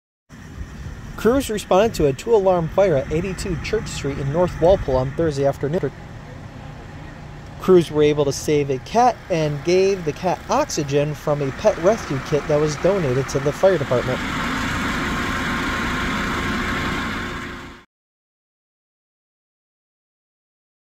Fire in North Walpole
Crews respond to a two-alarm fire at 82 Church Street in North Walpole, N.H., on Thursday, Sept. 21, 2023.